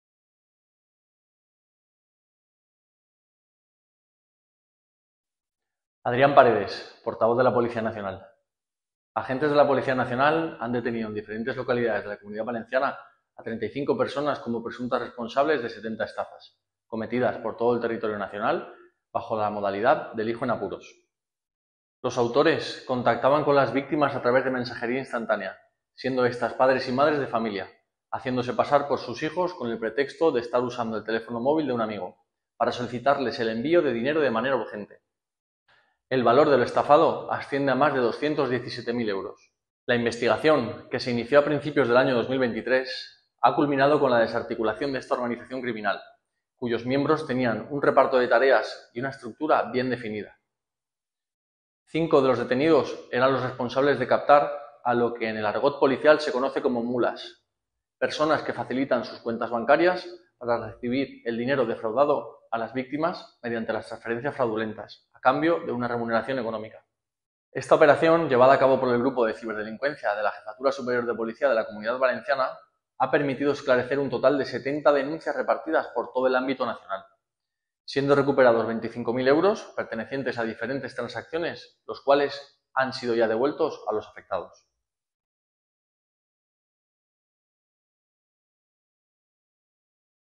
La Policía Nacional detiene a 35 personas por la comisión de estafas
La Policía Nacional detiene a 35 personas por la comisión de estafas mediante la modalidad del “hijo en apuros”